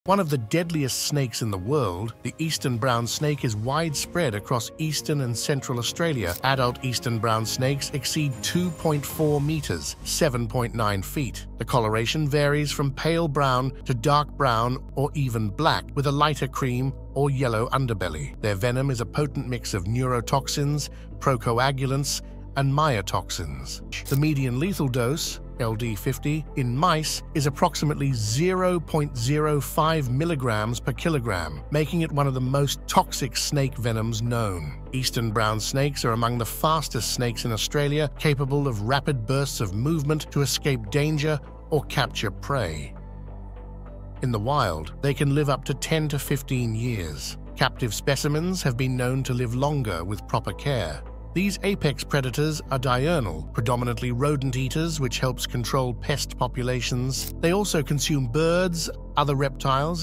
Australia's Deadliest Snake
Prepare to be both fascinated and terrified as we dive into the world of the Eastern Brown Snake, nature's very own ninja assassin! This slippery critter isn't just any snake—it's one of the most venomous creatures on Earth. Found in the wilds of Australia, the Eastern Brown Snake packs a venomous punch that makes even the bravest souls think twice.

In this video, we'll uncover the secrets of its stealthy lifestyle, its lightning-fast strikes, and its surprisingly sophisticated survival tactics. Want to know what makes this snake tick (or should we say slither)? We’ve got the inside scoop on its diet, hunting methods, and why it's considered both a hero and a villain in the ecosystem.

But don't worry, it's not all doom and gloom—we’ll throw in some lifesaving tips on what to do if you ever have a close encounter with this scaly daredevil. Get ready for a wild ride as we explore the beauty, danger, and downright jaw-dropping facts about the Eastern Brown Snake. Trust us, this is one adventure you won't want to miss!